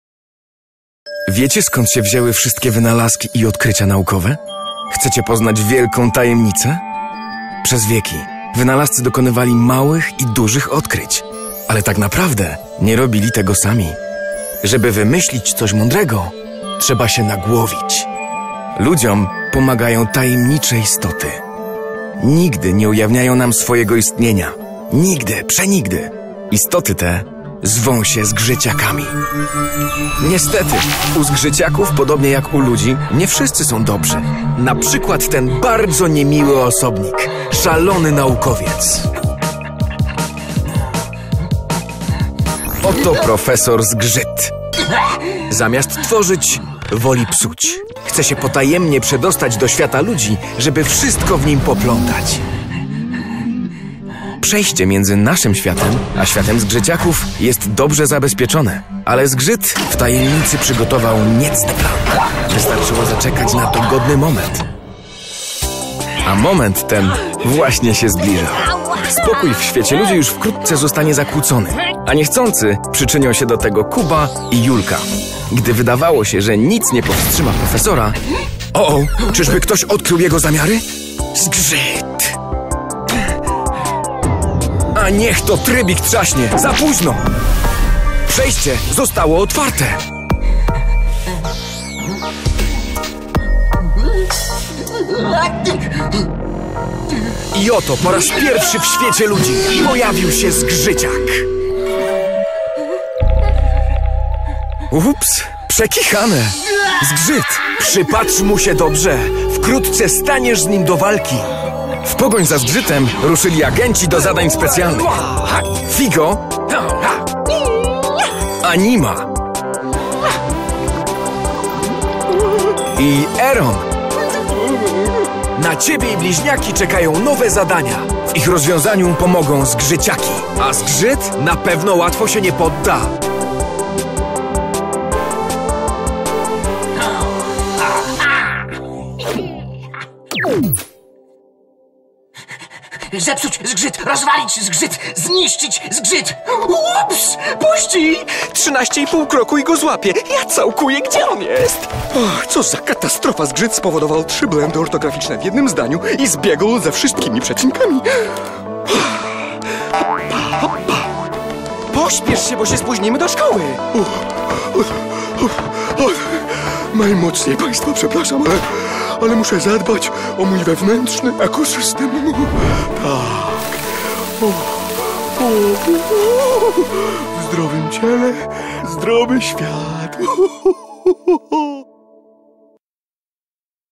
W świecie ludzi i w świecie Zgrzyciaków
Prezentacja koncepcji e-podręcznika do edukacji wczesnoszkolnej przygotowana w ramach projektu.